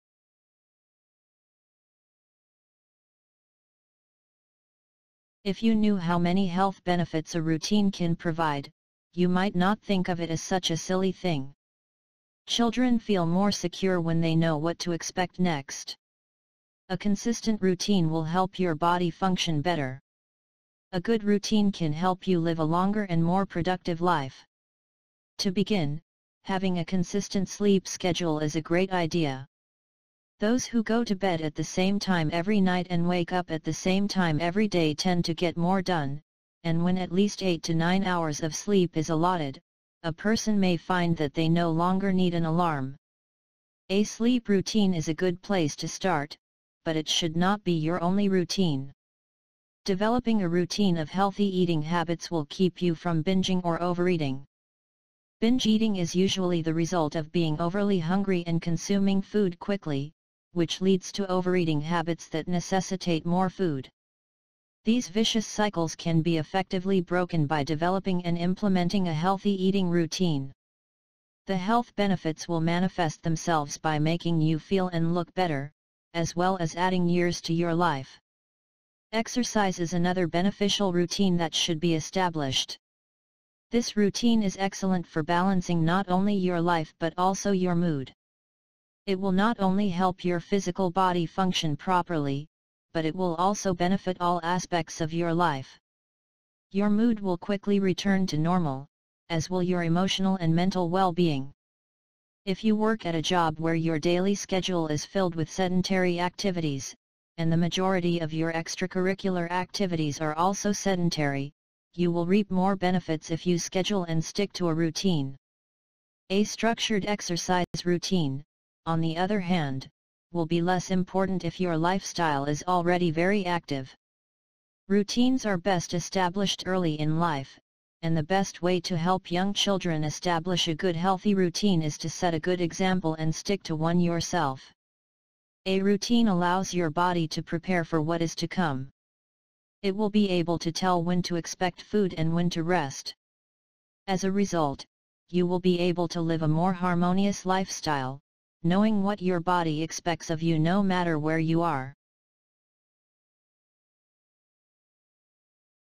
Routine Benefits That Will Change Your Life
To begin, having a consistent sleep schedule is a great idea. Those who go to bed at the same time every night and wake up at the same time every day tend to get more done, and when at least 8-9 hours of sleep is allotted, a person may find that they no longer need an alarm.

A sleep routine is a good place to start, but it should not be your only routine. Developing a routine of healthy eating habits will keep you from bingeing or overeating. Binge eating is usually the result of being overly hungry and consuming food quickly, which leads to overeating habits that necessitate more food.

Exercise is another beneficial routine that should be established. This routine is excellent for balancing not only your life but also your mood. It will not only help your physical body function properly, but it will also benefit all aspects of your life. Your mood will quickly return to normal, as will your emotional and mental well-being.

These vicious cycles can be effectively broken by developing and implementing a healthy eating routine. The health benefits will manifest themselves by making you feel and look better, as well as adding years to your life.

If you work at a job where your daily schedule is filled with sedentary activities and the majority of your extracurricular activities are also sedentary, you will reap more benefits if you schedule and stick to a routine. A structured exercise routine, on the other hand, will be less important if your lifestyle is already very active.

If you knew how many health benefits a routine can provide, you might not think of it as such a silly thing. Children feel more secure when they know what to expect next. A consistent routine will help your body function better. A good routine can help you live a longer and more productive life.

Routines are best established early in life, and the best way to help young children establish a good healthy routine is to set a good example and stick to one yourself. A routine allows your body to prepare for what is to come. It will be able to tell when to expect food and when to rest. As a result, you will be able to live a more harmonious lifestyle, knowing what your body expects of you no matter where you are.